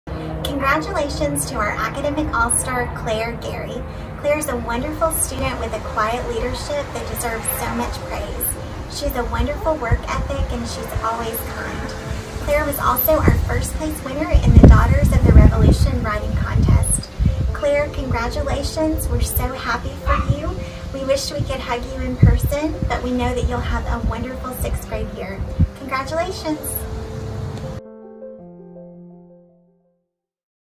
WSES Academic All-Star
Winstead Elementary School is proud to announce Claire Garry as the winner of this year's Academic All-Star Award!